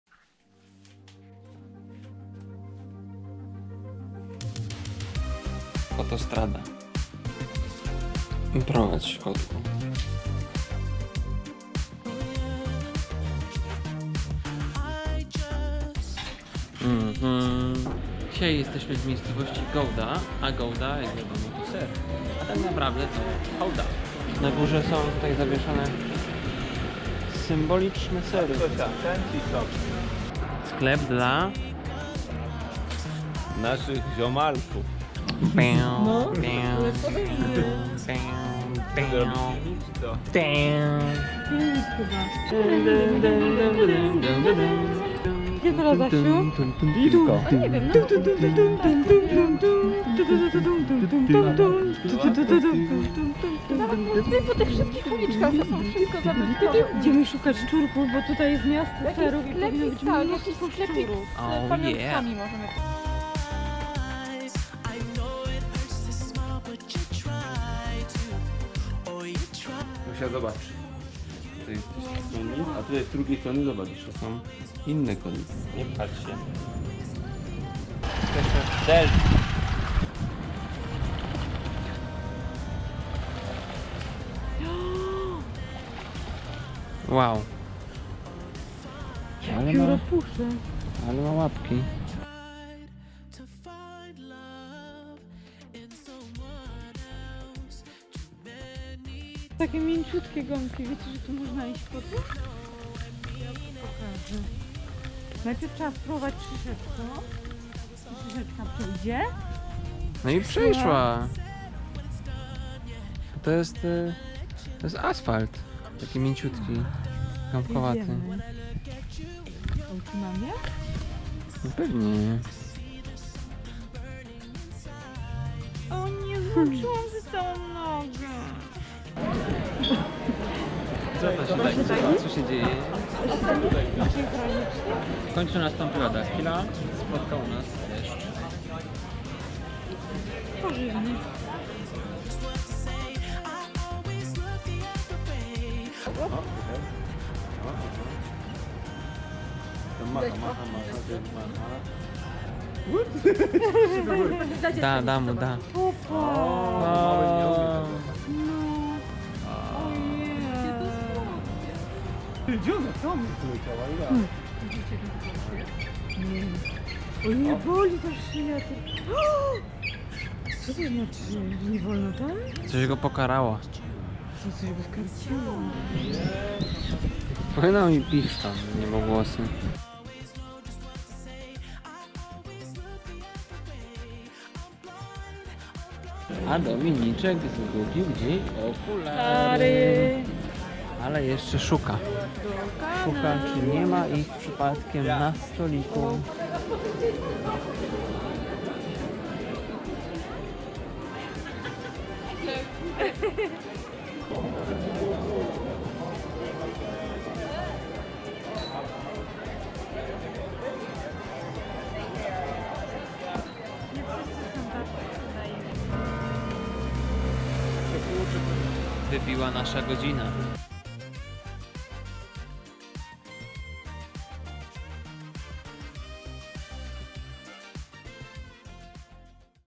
Niemcy i Holandia - cz. 7 - Gouda [4K]
Kolejne holenderskie miasto związane z nazwą sera - Gouda.